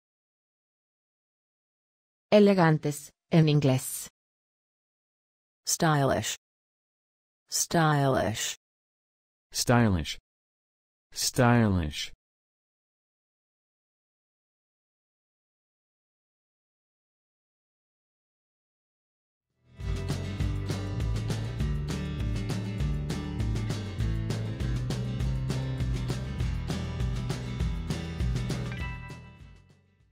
Cómo se dice elegantes en inglés
¿Cómo se dice correctamente elegantes en inglés? Escuche con nosotros.
¿Cuál es la traducción correcta de la palabra elegantes del español al inglés? Habla como los americanos. Además, ¿cómo escribir correctamente la palabra stylish?

¡Gracias!